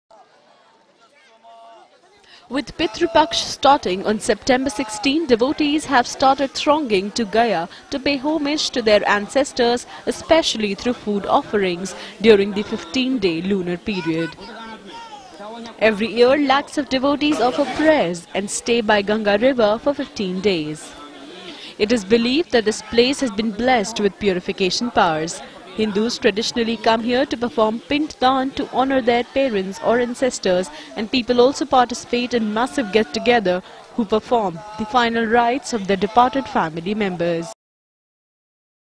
गया में पितरों को तर्पण देने पहुंचे लोग
16 सितंबर से पितृ पक्ष की शुरुआत होगी
गया में लोग अपने पूर्वजों का श्राद्ध कर उन्हें तर्पण देते हैं
यहां लोग पिंडदान के लिए भी बड़ी संख्या में आते हैं